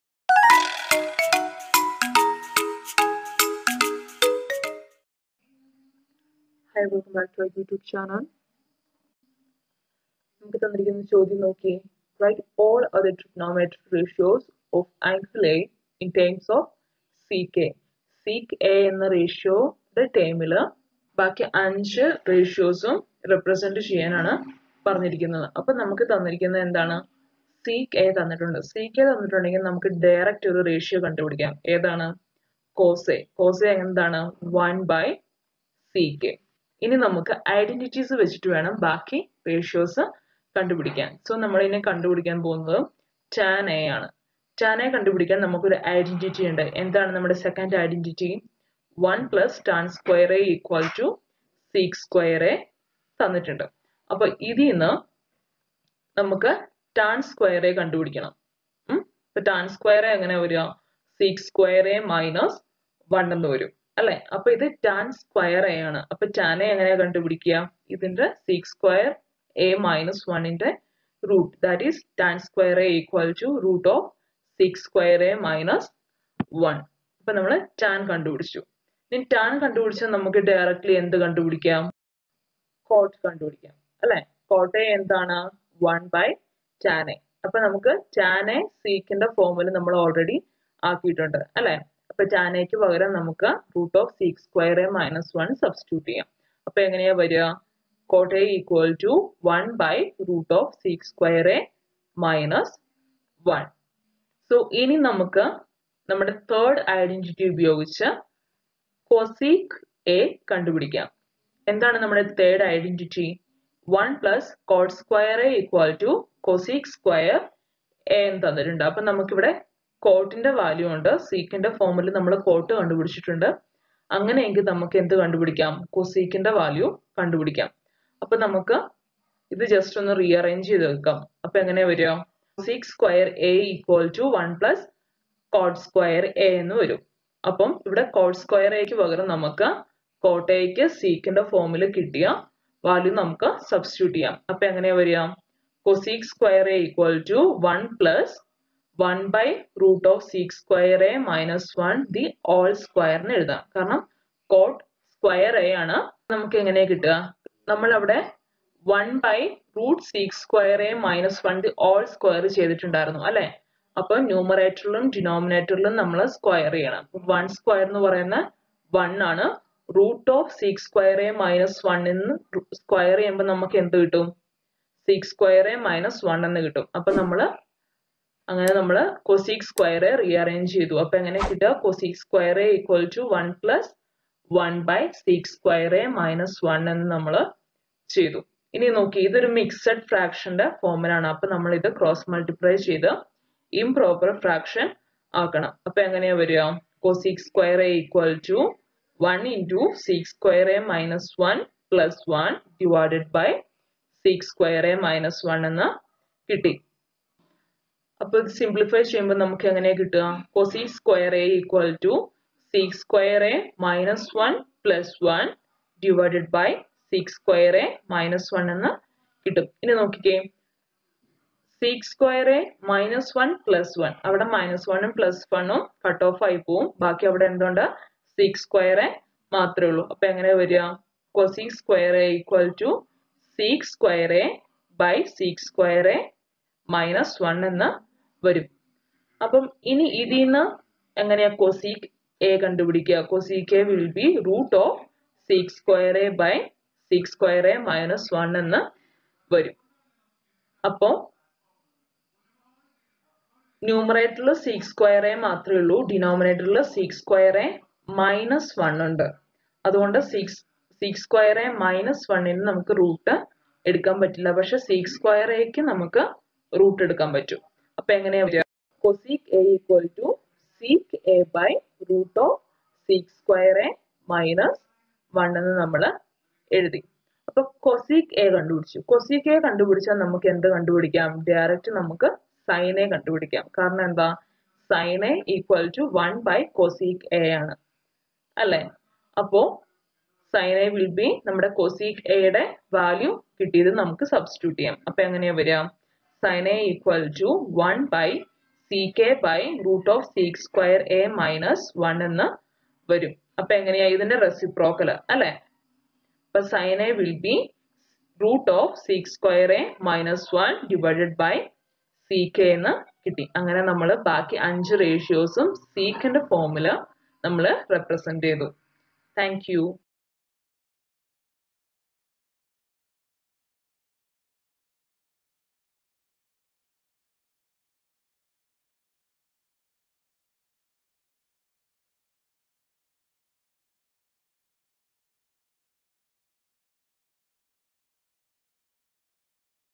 Write all the other trigonometric ratios of ∠A in terms of sec A || Trigonometry
Write all the other trigonometric ratios of ∠A in terms of sec A || Trigonometry || Expressing ratio in other ratios...

Trigonometric Identities - Short cut method to remember identities